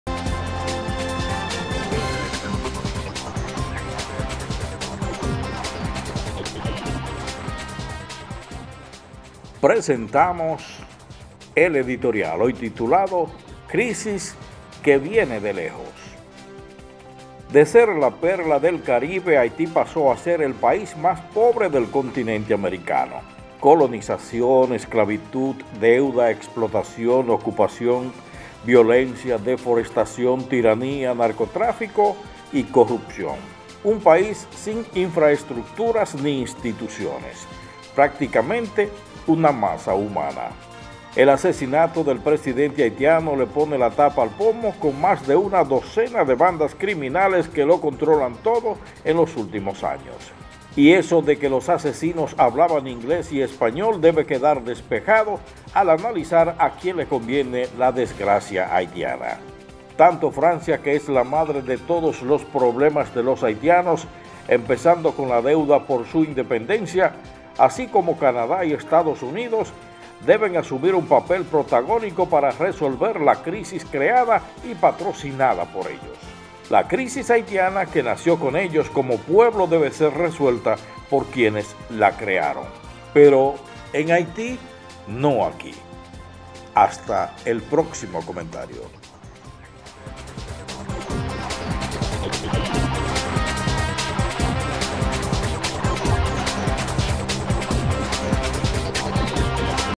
Crisis que viene de lejos
De ser la perla del Caribe Haití pasó a ser el país más pobre del continente.

Colonización, esclavitud, deuda, explotación, ocupación, violencia, deforestación, tiranía, narcotráfico y corrupción.

Un país sin infraestructuras ni instituciones, prácticamente una masa humana.

El asesinato del presidente haitiano le pone la tapa al pomo con más de una docena de bandas criminales que lo controlan todo en los últimos años.

Y eso de que los asesinos hablaban inglés y español debe quedar despejado al analizar a quién le conviene la desgracia haitiana.

Tanto Francia, que es la madre de todos los problemas de los haitianos empezando con la deuda por la independencia, así como Canadá y Estados Unidos deben asumir un papel protagónico para resolver esa crisis creada y patrocinada por ellos.

La crisis haitiana, que nació con ellos como pueblo, debe ser resuelta por quienes la crearon.

Pero en Haití, no aquí.